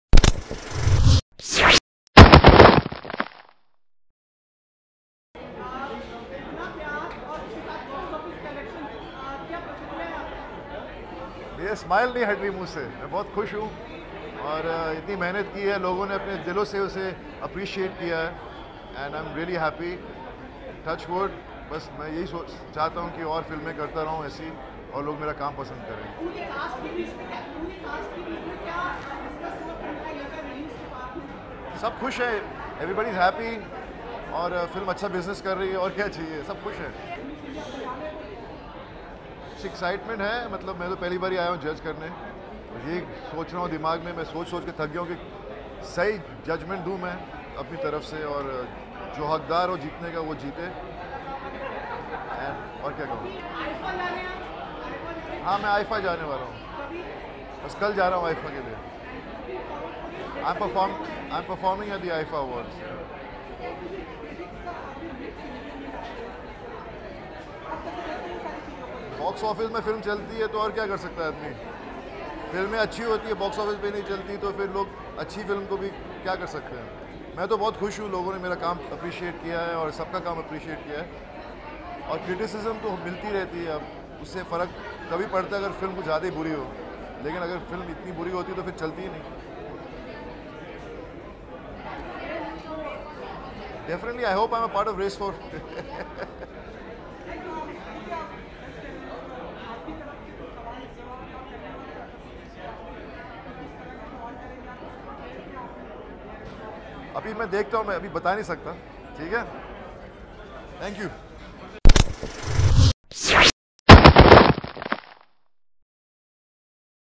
बॉबी बने सलमान भक्त, कहीं भी जाओ सलमान के गुण गाओ
Bobby Deol PRAISES Salman Khan For casting him in Race 3.